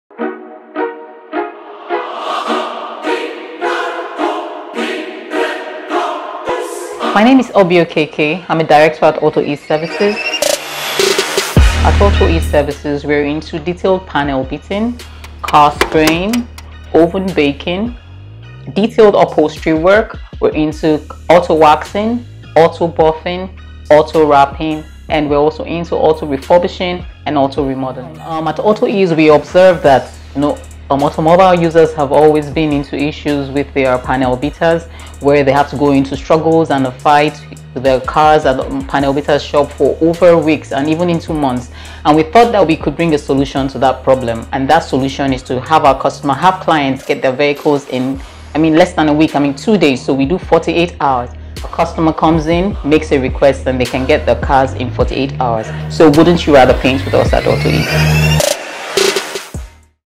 AutoEase Services Ad - A Blueafric Production
Autoease is a state of the art facility, dedicated to providing customers with outstanding service delivery and quality autoboost experience.
Services Includes:
Oven-Baked Automotive Painting
Full/Partial Re-sprays
Panel Beating/Bumper Repairs & Welding
Scratch & Dent Repairs
Chassis Straightening & Accidented Work
Car Upholstery & Interior Restoration
Auto-buffing, Customization & Upgrading